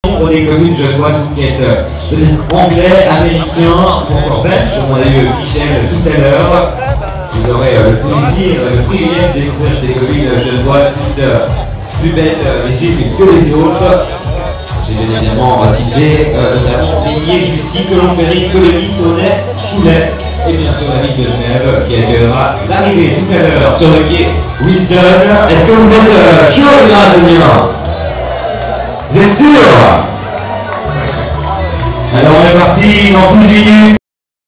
DSCN0912 - Marathon de Genève 15 Mai 2011
Départ: Chêne-Bourg

Préparatifs au départ de la course. Avenue de Bel-Air.